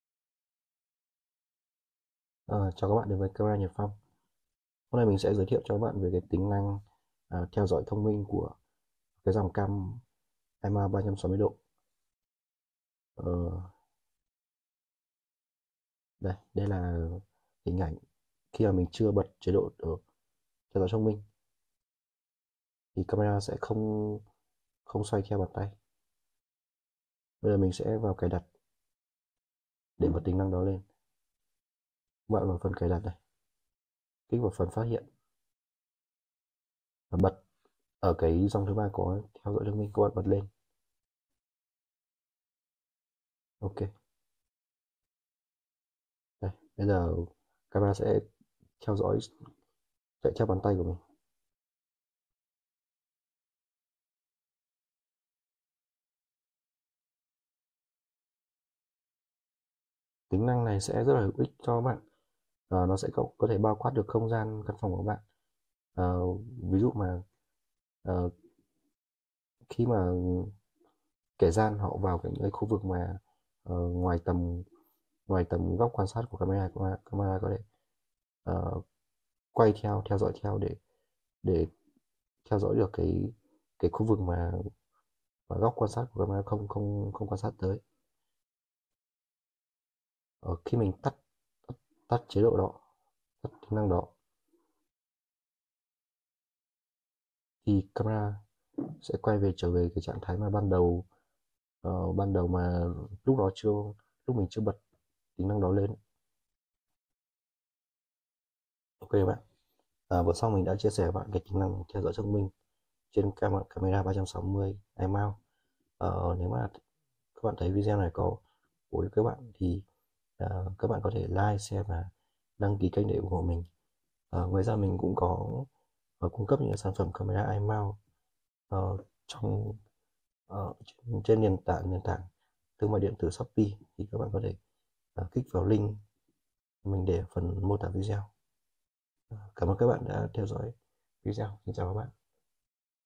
Hướng dẫn bật tính năng theo dõi thông minh cho camera xoay 360 độ Imou - Camera Nhật Phong
- Hướng dẫn bật tính năng theo dõi thông minh cho camera xoay 360 độ Imou
- 1 số tính năng nổi bật của camera không dây Imou A22EP:
+ Độ phận giải 2Mp, độ nét Full HD, 1080p
+ Xoay 360 độ đảm bảo không có điểm mù trong phòng của bạn
+ Phát hiện chuyển động con người và gửi thông báo về điện thoại
+ Theo dõi chuyển động
+ Đàm thoại 2 chiều
+ Lưu trữ đa dạng: Thẻ nhớ, đám mây, đầu ghi NVR
+ Hú còi báo động
- Cam kết hàng chínhh hãng, mới 100%, được bảo hành 2 năm